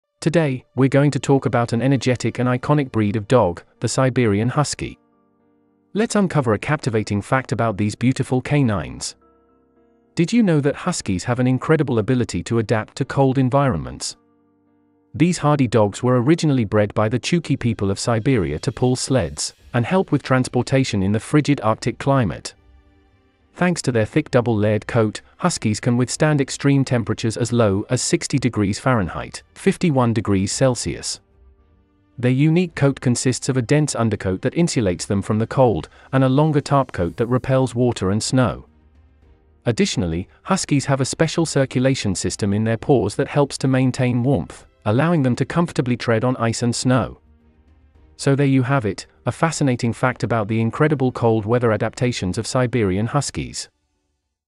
The Siberian Husky - Incredible cold-weather adaptations
Today, we're going to talk about an energetic and iconic breed of dog—the Siberian Husky. Let's uncover a captivating fact about these beautiful canines!

Did you know that Huskies have an incredible ability to adapt to cold environments? These hardy dogs were originally bred by the Chukchi people of Siberia to pull sleds and help with transportation in the frigid Arctic climate. Thanks to their thick double-layered coat, Huskies can withstand extreme temperatures as low as -60°F (-51°C). Their unique coat consists of a dense undercoat that insulates them from the cold, and a longer topcoat that repels water and snow. Additionally, Huskies have a special circulation system in their paws that helps to maintain warmth, allowing them to comfortably tread on ice and snow.

So there you have it—a fascinating fact about the incredible cold-weather adaptations of Siberian Huskies.